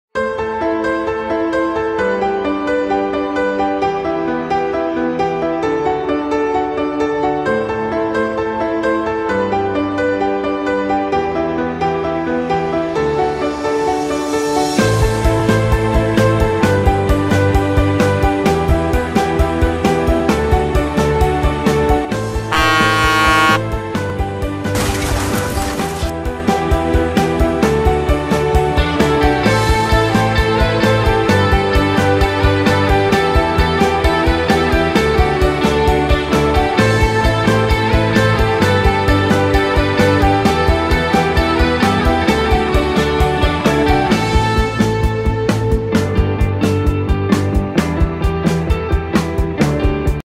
We throw the cars off the cliff
we throw toy cars off a cliff, with the help of a truck.